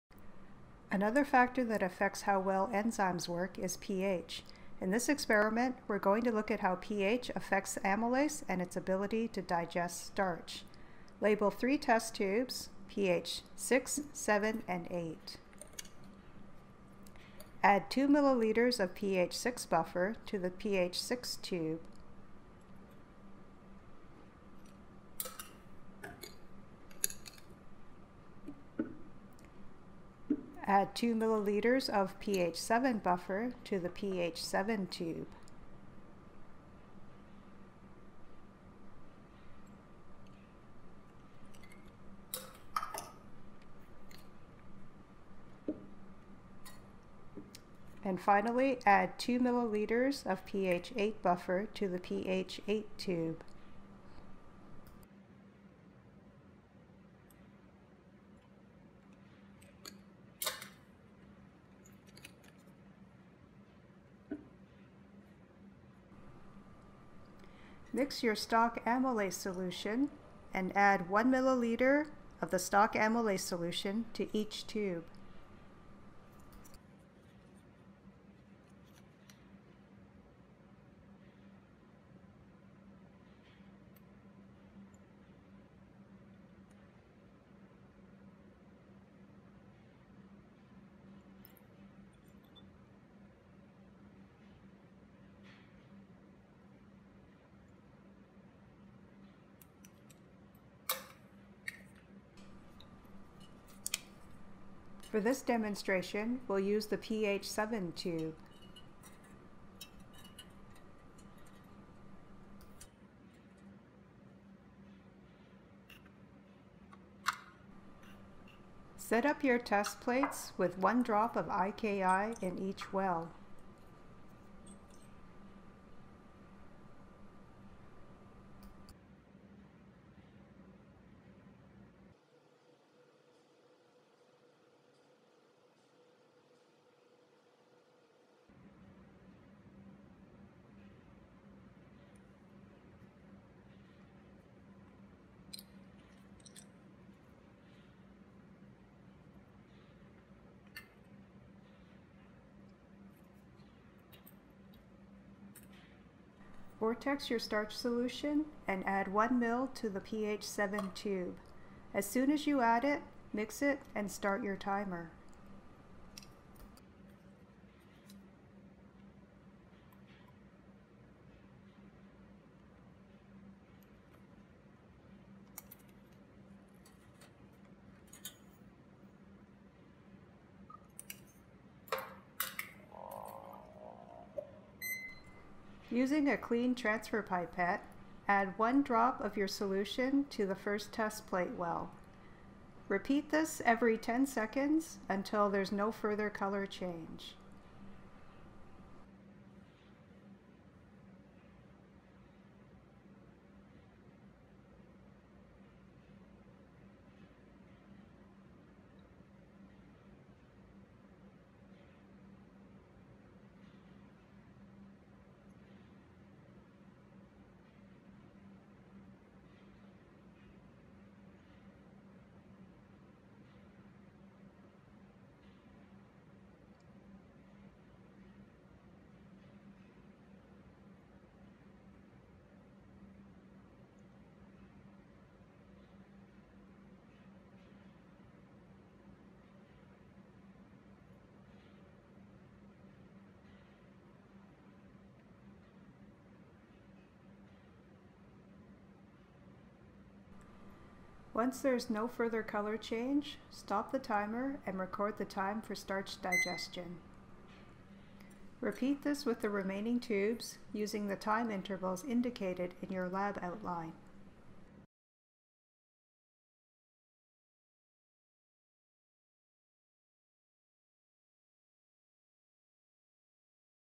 BIO202 Digestion Lab Part 2 Effect of pH on Starch Digestion
BIO202 - Introductory Animal Physiology

This lab demonstrates the effect of pH on the ability of amylase to digest starch.

Lisa Cheung and Randy Preising
Department of Biology
University of Toronto Mississauga